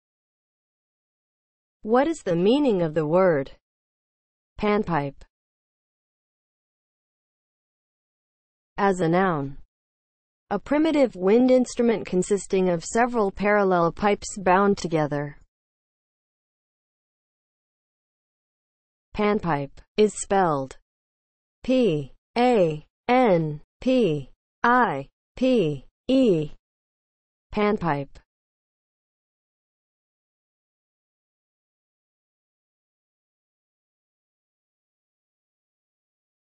What is the meaning of the word PANPIPE?
Definition and spelling of the word PANPIPE.

(00:00) Intro
(00:07) As a noun
(00:17) Spelling

As a noun (00:07):
• a primitive wind instrument consisting of several parallel pipes bound together